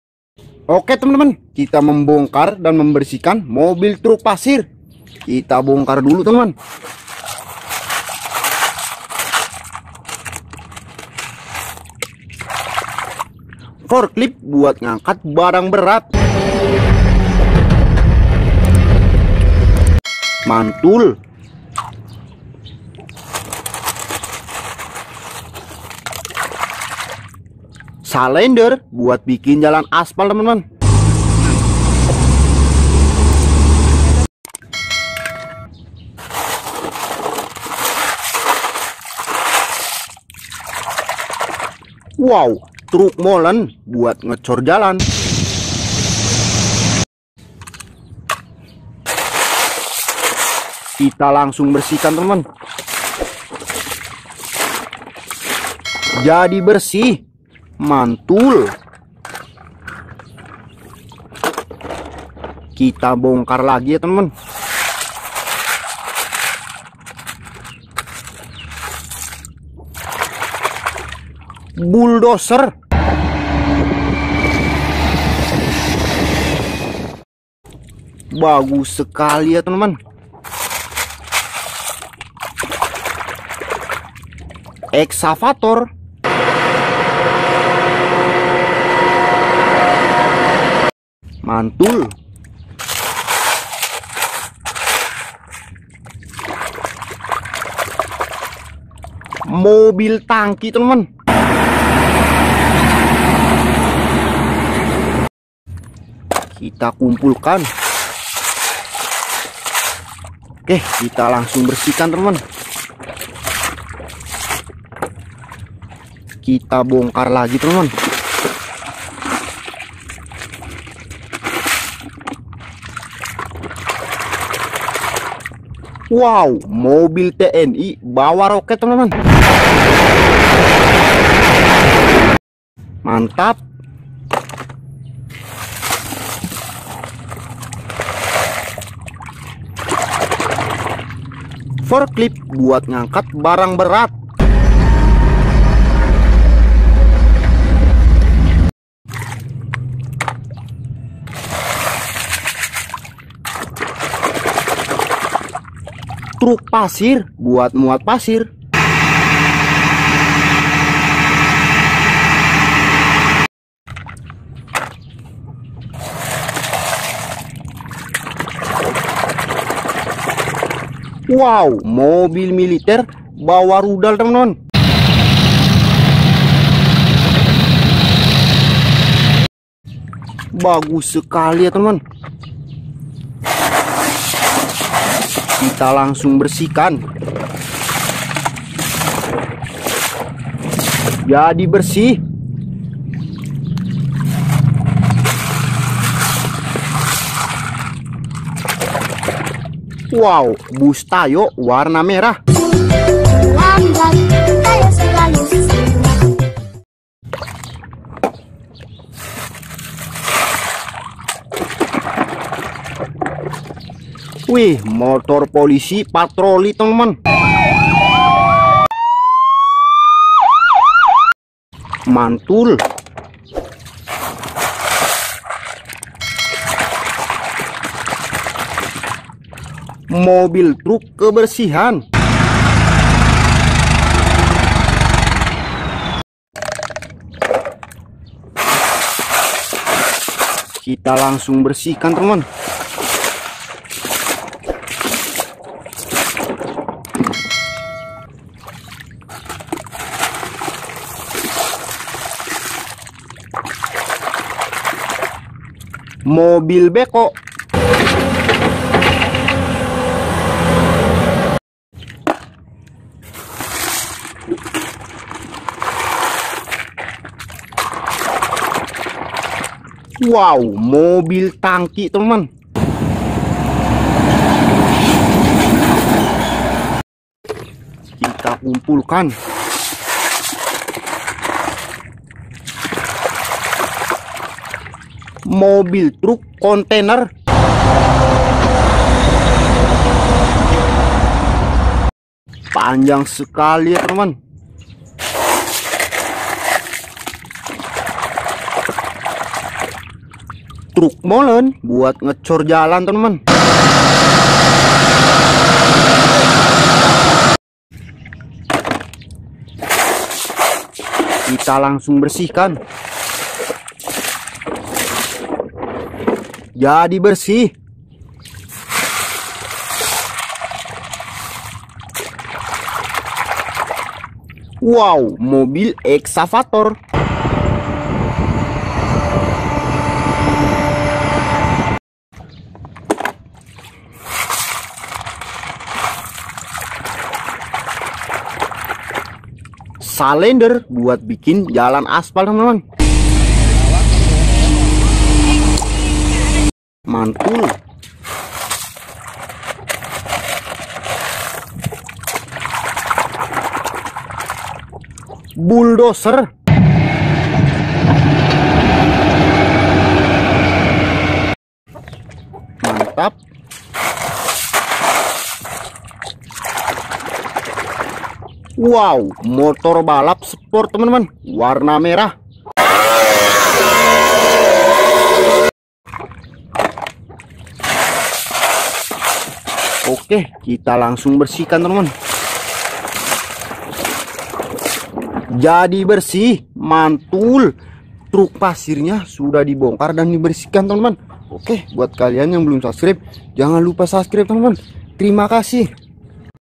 Membersihkan Mobil Truk Pasir Dan Menemukan Truk Molen, Stum Slender, Forklift, Mobil Militer
Membersihkan Mobil Truk Pasir Dan Menemukan Truk Molen, Stum Slender, Forklift, Mobil Militer, Mobil Tangki